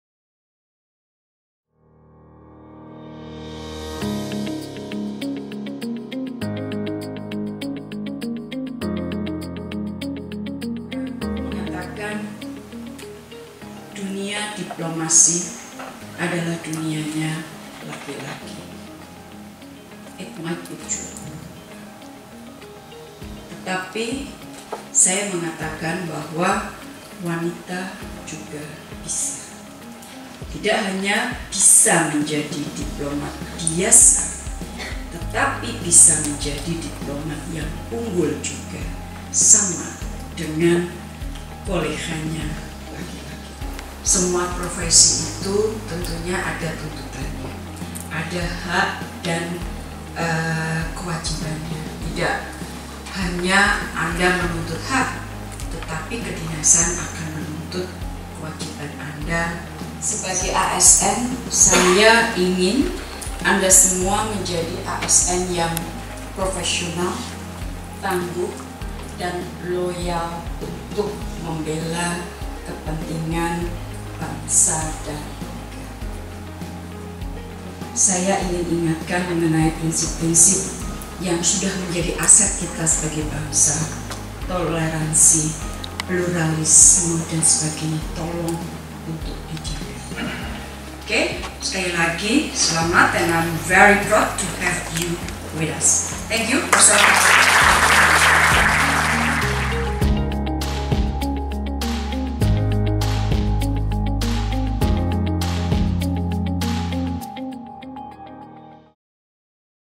Menlu Retno kepada CPNS Kemlu: Jadilah ASN Tangguh, Professional, Jaga Toleransi dan Pluralisme.
Menteri Luar Negeri, Retno Marsudi menerima secara resmi Calon Pegawai Negeri Sipil Kementerian Luar Negeri Indonesia di Pusat Pendidikan dan Pelatihan Kemlu RI (8/2).

Dalam sambutannya, Menlu menekankan perlunya CPNSKemlu untuk menjaga profesionalisme dan tangguh untuk menjawab berbagai tantangan pekerjaan. Pegawai Kemlu juga harus menjaga Toleransi dan Pluralisme sebagai nilai nilai bangsa.

Selamat datang dan selamat bergabung para rekan-rekan CPNS Kemlu! Selamat berkarya dan berdiplomasi.